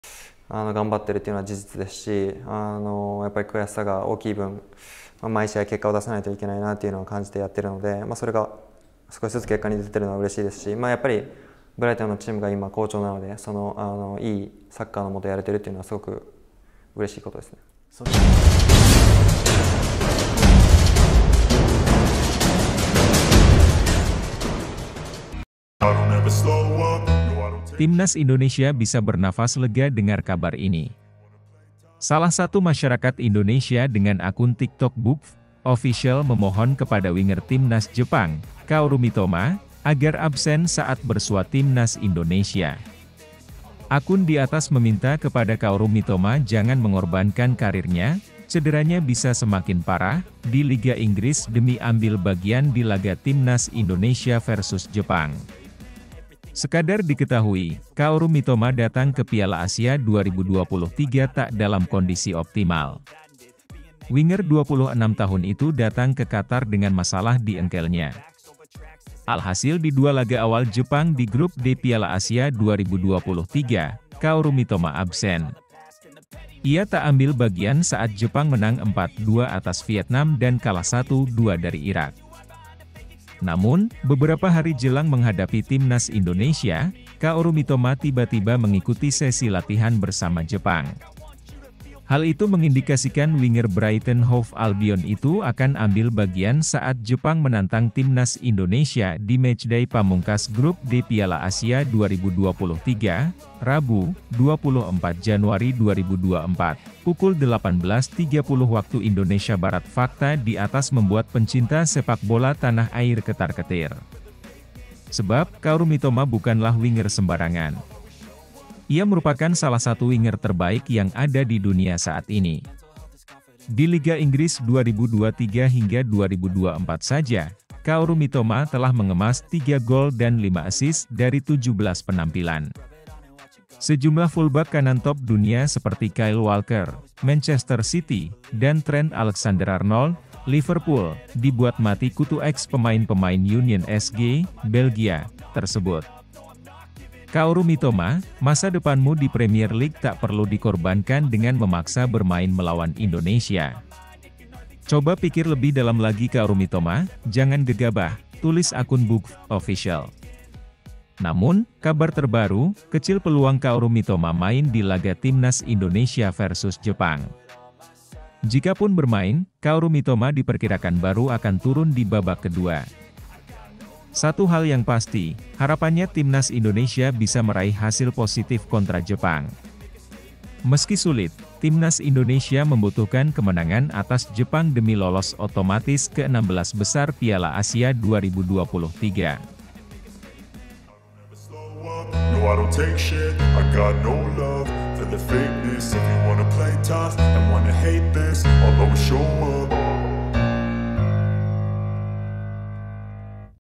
🔵 DAPAT KEUNTUNGAN BESAR !! Timnas Indonesia Bisa Coba Memanfaatkan Hal Ini Untuk Kalahkan Jepang
BERITA BOLA TERBARU DAN TERUPDATE DARI TIMNAS UPDATE - Berita Bola Hari Ini :

Kaoru Mitoma datang ke Piala Asia 2023 tak dalam kondisi optimal. Winger 26 tahun itu datang ke Qatar dengan masalah di engkelnya. Alhasil di dua laga awal Jepang di Grup D Piala Asia 2023, Kaoru Mitoma absen. Ia tak ambil bagian saat Jepang menang 4-2 atas Vietnam dan kalah 1-2 dari Irak. Namun, beberapa hari jelang menghadapi Timnas Indonesia, Kaoru Mitoma tiba-tiba mengikuti sesi latihan bersama Jepang. Hal itu mengindikasikan winger Brighton & Hove Albion itu akan ambil bagian saat Jepang menantang Timnas Indonesia di matchday pamungkas Grup D Piala Asia 2023, Rabu (24/1/2024) pukul 18.30 WIB. Fakta di atas membuat pencinta sepakbola Tanah Air ketar-ketir. Sebab, Kaoru Mitoma bukanlah winger sembarangan. Ia merupakan salah satu winger terbaik yang ada di dunia saat ini. Di Liga Inggris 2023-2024 saja, Kaoru Mitoma telah mengemas tiga gol dan lima assist dari 17 penampilan. Sejumlah fullback kanan top dunia seperti Kyle Walker (Manchester City) dan Trent Alexander-Arnold (Liverpool) dibuat mati kutu eks pemain pemain Union SG (Belgia) tersebut.

Tonton videonya hingga selesai untuk lebih memahami isi konten selengkapnya. Tulis dikolom komentar jika ada saran untuk channel ini.